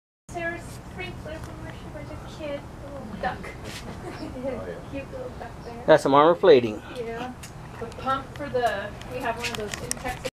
Jeep Grand Cherokee ZJ 5.9 Fuel Tank Armor Installed
To help mitigate a possible fuel tank fire on my ZJ, I installed Fuel Tank Armor made by Rock Hard 4X4.